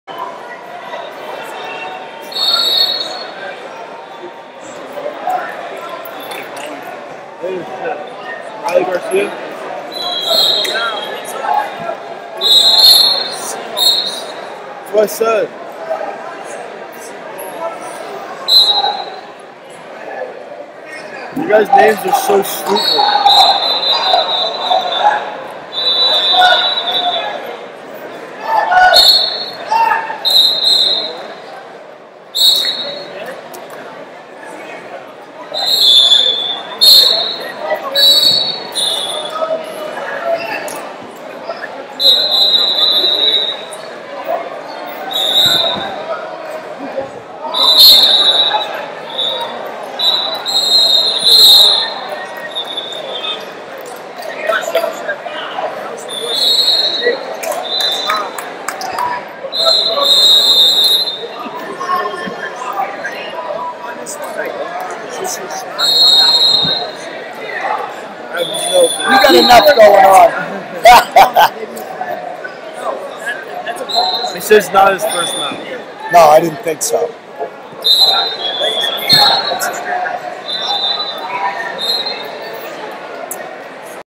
160 Lbs Round Of 16 - Aiden Gaugler, Pennsylvania Vs Reily Garcia, New Jersey 5845
2022 Phil Portuese Northeast Regional Championships - 160 Lbs Round Of 16 - Aiden Gaugler, Pennsylvania Vs Reily Garcia, New Jersey 5845